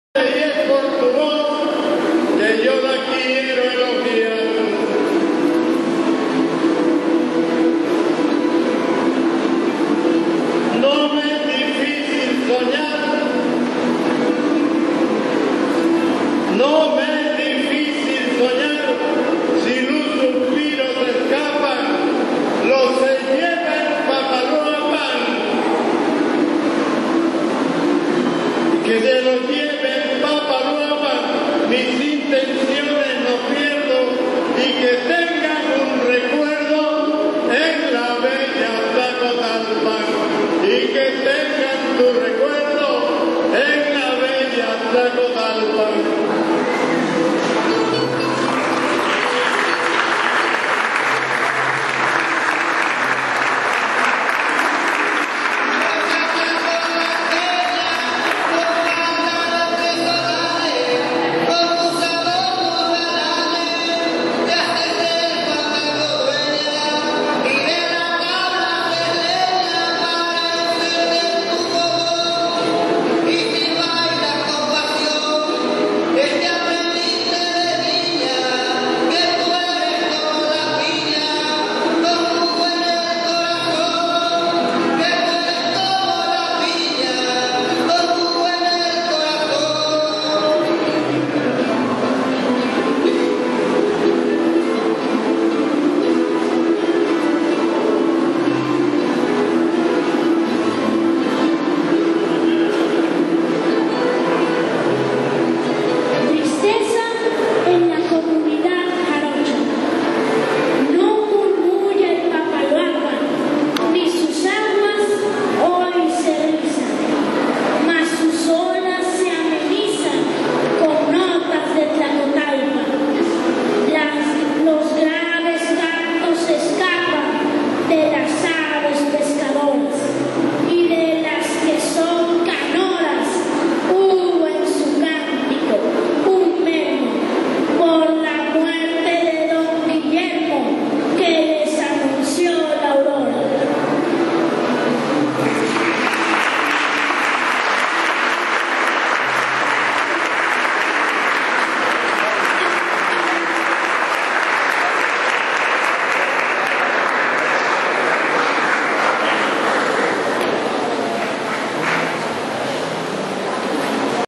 hasta pronto tio guillo.wmv
El Dr. Rodrigo gutierrez castellanos, se presento en la capilla del IVEC de este puerto de veracruz, el pasado, 18 de marzo del presente, para acompañar a los familiares, en su dolor y a su vez a recordar a su amigo el Lic. Guillermo  Chazaro lagos, a quie le canto y verso. tambiem presento al Niño, Abraham Gutierrez barreto, para que le diera voz a su letra.